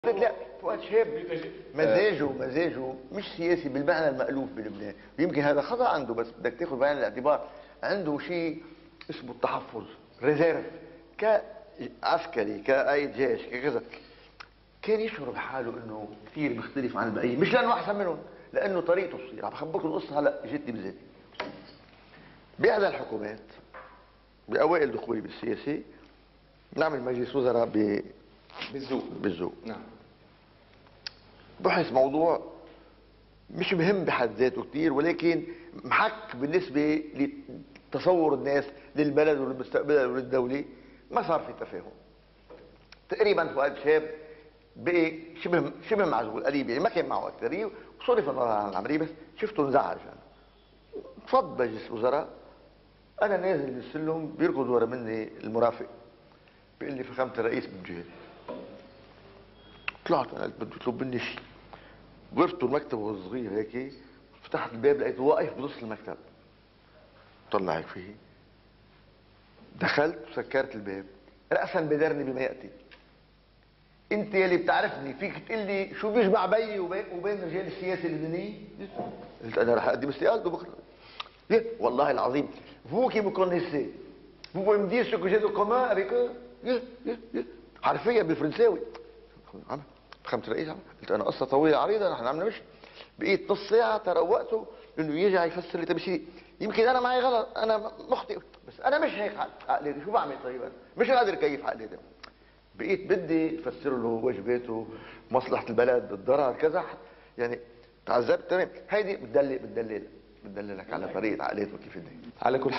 Fouad Boutros on Fouad Chehab and the Lebanese traditional politicians (LBC - 1998)
Prominent Lebanese statesman and diplomat, Minister Fouad Boutros talks about the late Lebanese President Fouad Chehab (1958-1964) and his relationship with the traditional Lebanese politicians (LBC talkshow - 1998).